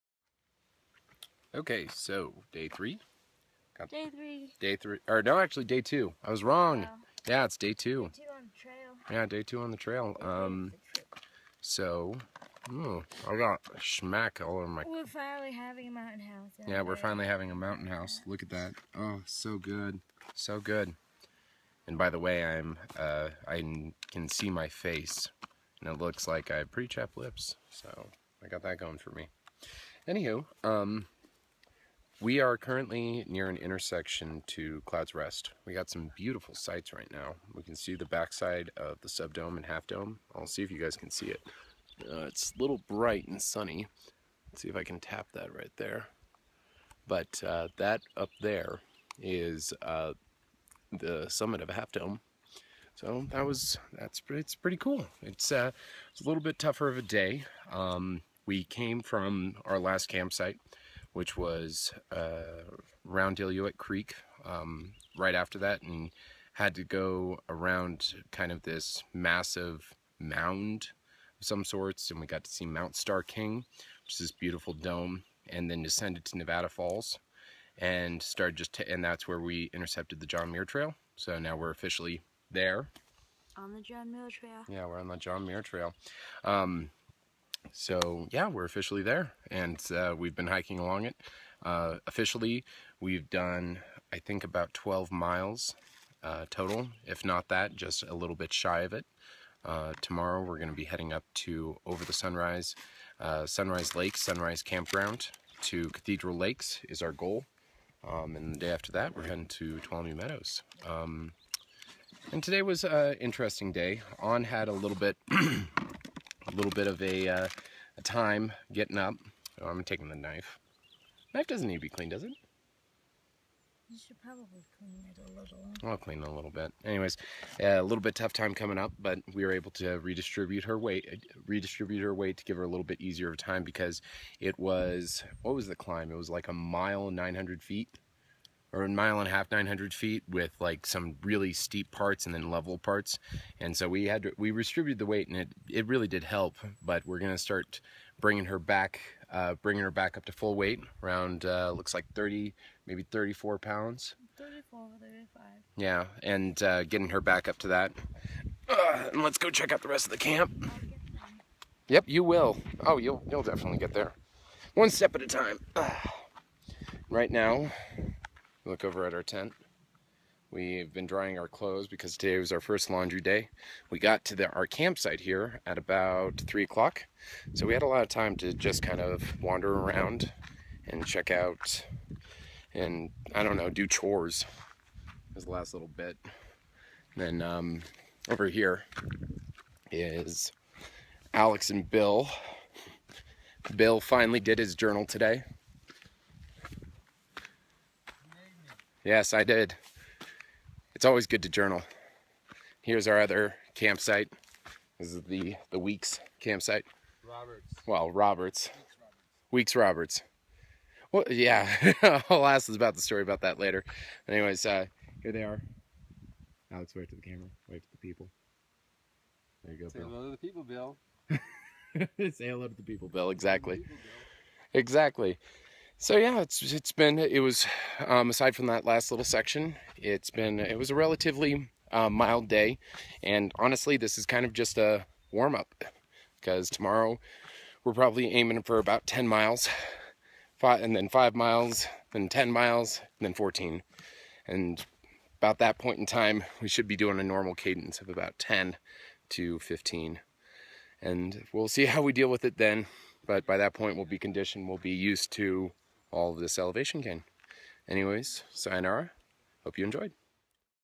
Fixie's JMT Video Blog - Day 2
7.3 miles, Illilouette Creek to Cloud's Rest Trail Intersection.

Highlights:
Nevada Falls
Little Yosemite Valley
Half Dome Views